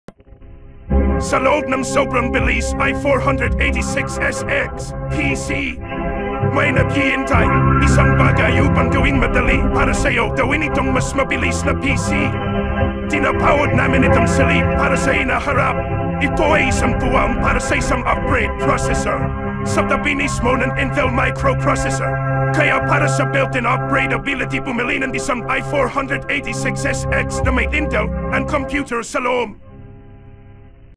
Intel i486SX Commercial #1 (1992 - Uka Uka Tagalog Filipino - BCC Videoscope)
Denoising, Deblocking, pag-alis ng compression artefact at upscaling gamit ang AI (BCC Videoscope)